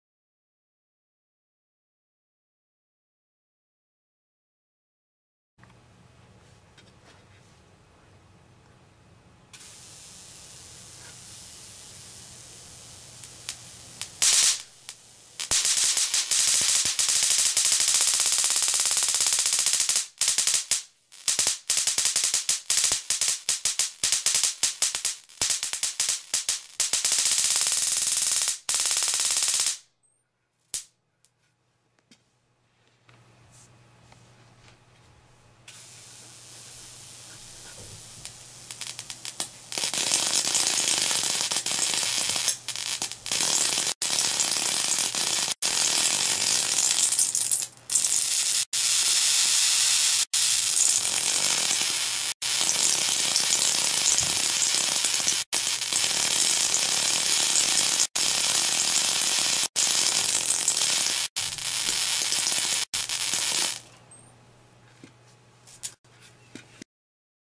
Voltage Multiplier (Running from a ZVS Driver)
The multiplier itself is capable of roughly 120kv however I'm not getting the desired output voltage because the capacitors are not getting fully charged from an inadequate power supply. This is why it's falling short of the 120kv.

Hopefully when I fix or get a new power supply, I will be able to take some fluoroscopic pictures (x-ray images) of various items using the dental intensifying screen.